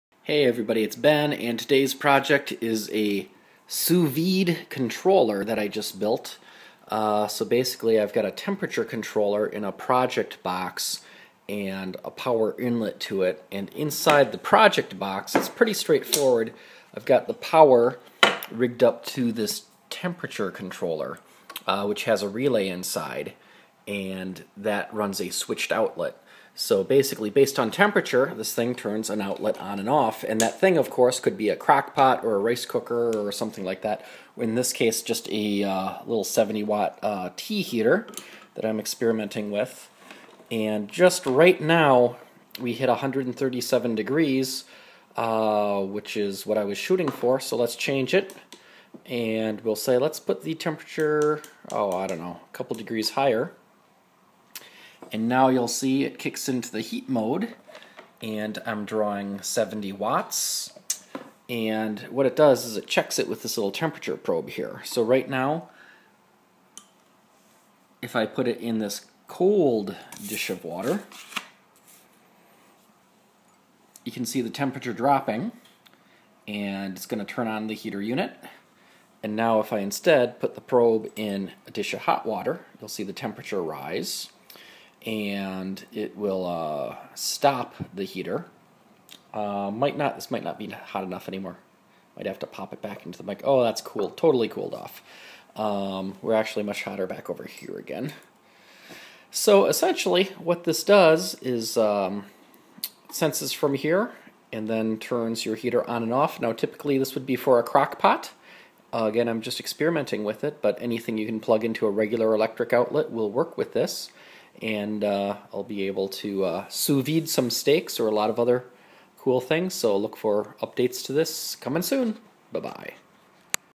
DIY Sous-Video MK2 show and tell
Just a quick look at my basic DIY Sous-Vide setup. It's a temperature controller, power inlet, and power outlet. The temperature controller has a 10 amp internal relay and turns the electric outlet on and off based on temperature. By switching power on/off to a Crock-Pot or other heater, any temperature can be chosen and held to within one degree F.